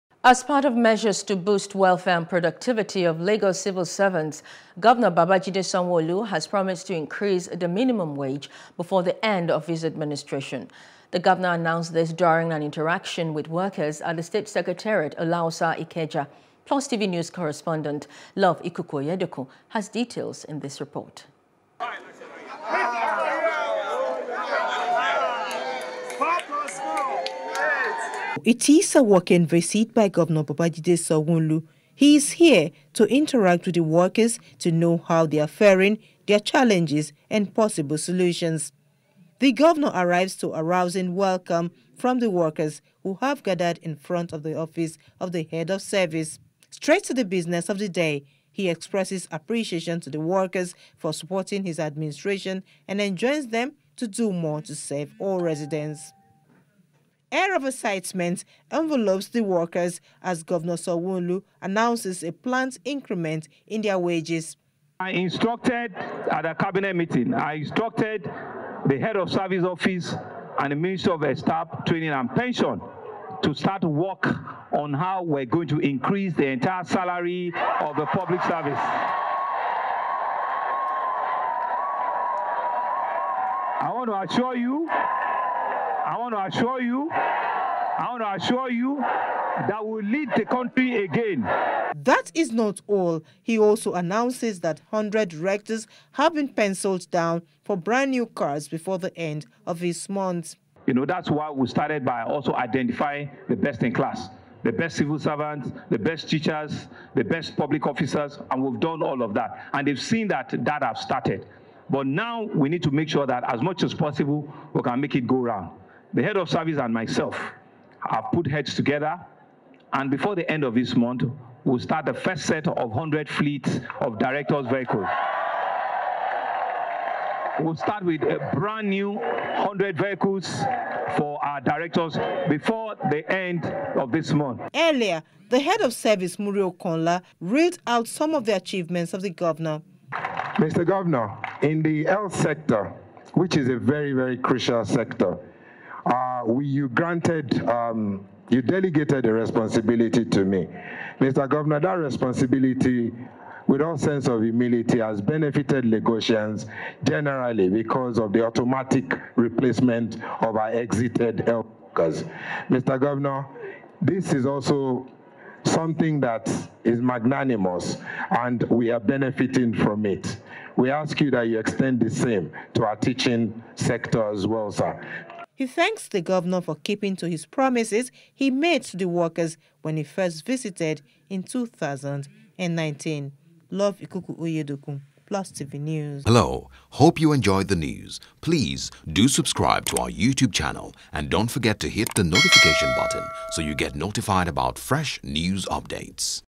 Lagos Civil Service: Sanwo-Olu Announces Salary Increment For Workers | NEWS
Welcome To The Plus Sports